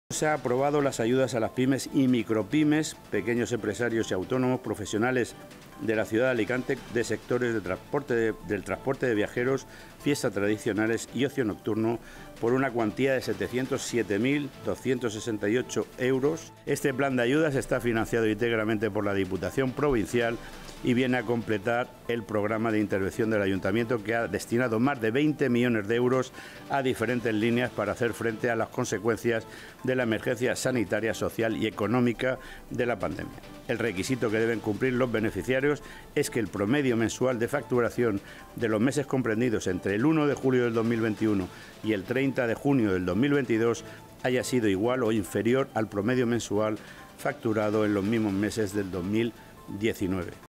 Alicante tramita 707 000 euros en ayudas al transporte, las fiestas y el ocio nocturno por los daños
La Junta de Gobierno Local ha aprobado este martes el Plan de Ayudas Diputación-Ayuntamiento 2022 por el que la concejalía de Empleo y Fomento gestionará en las próximas semanas la concesión de 707.268 euros en concepto de subvenciones a pymes, micropymes, autónomos y profesionales de los sectores del transporte de viajeros, fiestas tradicionales y ocio nocturno de la ciudad de Alicante para paliar los daños causados por la pandemia del covid-19.

Esta línea de ayudas, íntegramente financiada por la Diputación Provincial, contempla el requisito de que los potenciales beneficiarios deben acreditar un promedio mensual de facturación entre el 1 de julio de 2021 y el 30 de junio de 2022 igual o inferior a la media mensual de facturación durante los mismos meses de 2019.

El Ayuntamiento y la Diputación "se alinean en este compromiso necesario de minimizar el grave impacto del covid-19" sobre los sectores económicos de la ciudad.

Los tres sectores económicos beneficiarios de esta convocatoria son los del transporte de viajeros, las fiestas tradicionales y el ocio nocturno, en los que "se han constatado pérdidas importantes en el período comprendido entre el 1 de julio de 2021 al 30 de junio de 2022", según recoge el acuerdo de la Junta de Gobierno.

En concreto, sobre el primero de los sectores, el plan de ayudas Ayuntamiento-Diputación se refiere a las consecuencias sobre los profesionales del transporte de las "restricciones a la movilidad y desplazamientos turísticos internacionales que todavía existían hasta prácticamente mayo de este año, disminuyendo así de forma destacada la llegada de turistas extranjeros. A ello se une en este sector el problema del incremento exponencial del precio del combustible".

En cuanto al sector de fiestas tradicionales, el programa de ayudas reseña que hasta el pasado mes de marzo se mantuvo la prohibición de los festejos tradicionales, populares y masivos. Y sobre el ocio nocturno, alude a las restricciones que seguían vigentes para luchar contra el covid "como limitación de aforos, baile en la pista, uso de mascarillas en interiores,... que provocaron especialmente un aluvión de cancelaciones de encuentros de empresas, familiares, fiestas multitudinarias, etcétera, especialmente en el período navideño 2021-22"